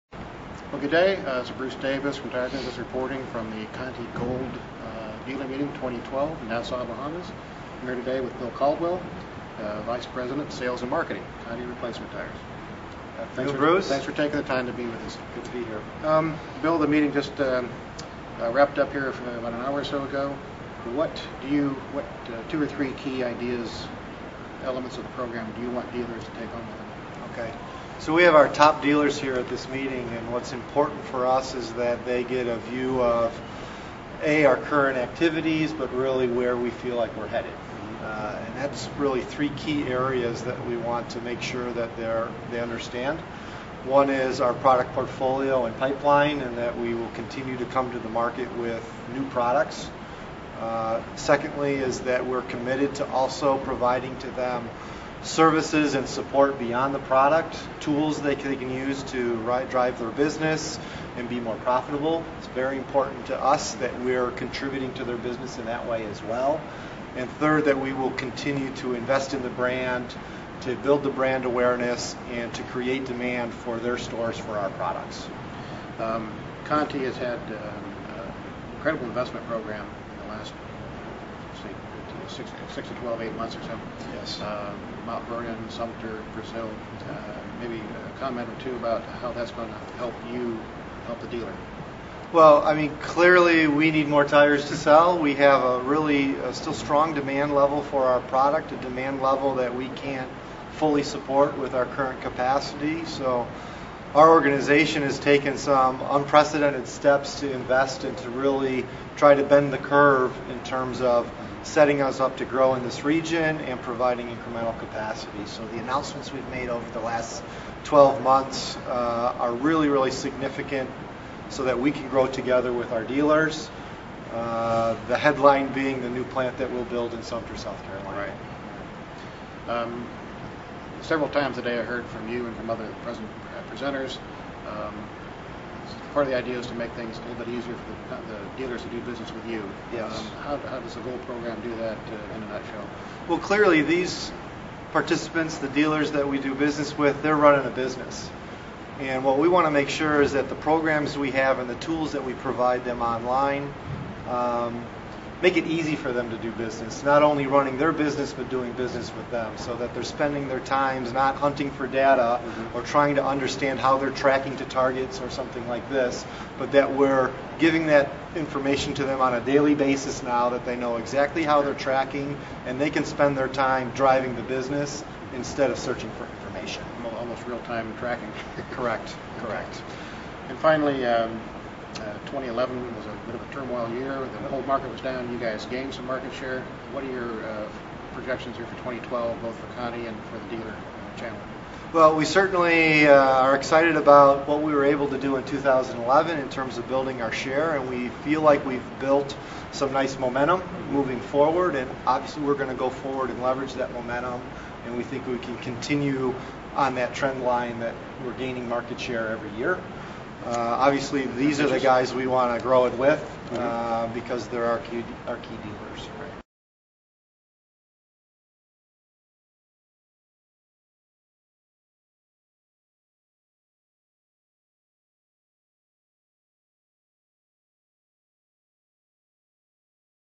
Continental Interview
In an exclusive interview with Tire Business, Continental's Bill Caldwell explained the company's sales and marketing strategies.